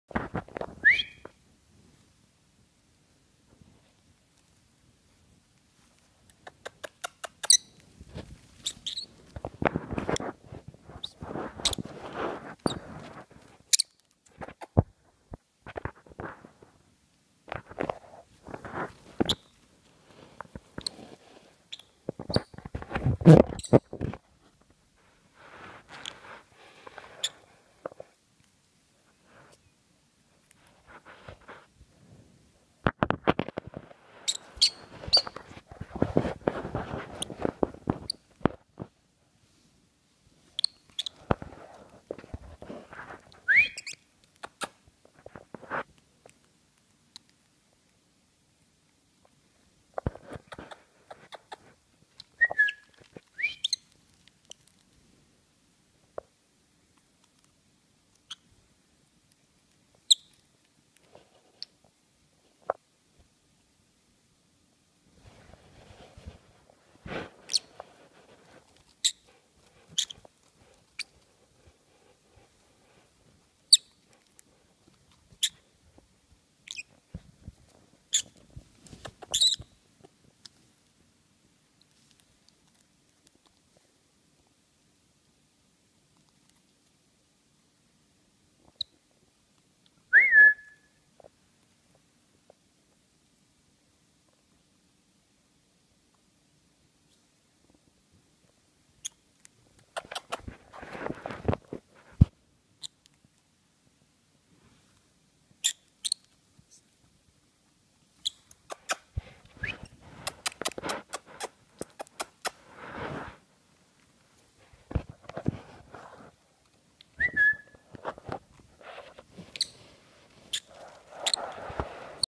Mr. peeps under covers playing
Mr. Peeps havin' a good time playing under covers!
I think it was pretty cute, but that's your decision.

This adorable little avian was found in a tree near our house. We called it down and tried to find an owner, but no luck. Now hes ours!.